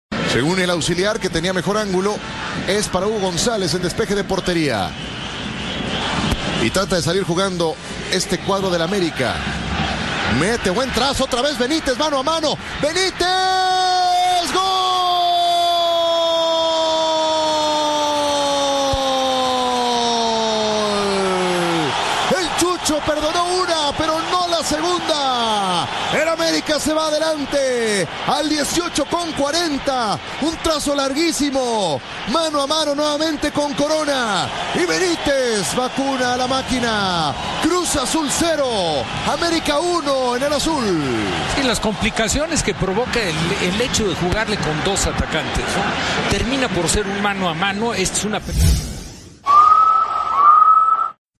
Águila Retro | Gol de Christian Benítez a Cruz Azul | Apertura 2012 | Águila en linea
RECORDABAS EL GOL?
                              ¡Disfrútalo!